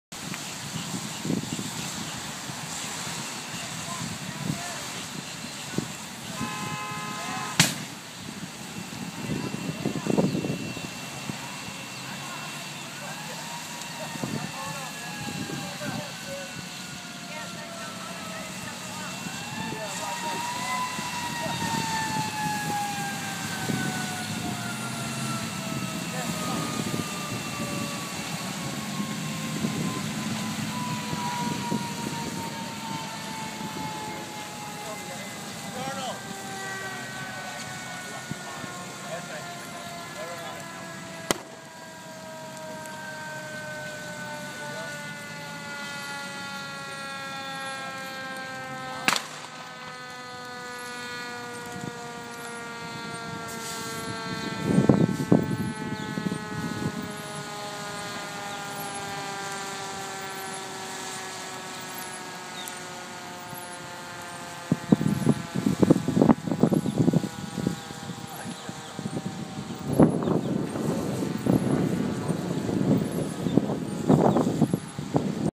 Ford expedition on fire in Merritt Island
Small explosions before firefighters arrive on scene. Driver escaped unharmed. Fire started shortly after driver started his car. He believes it was due to an ignition wire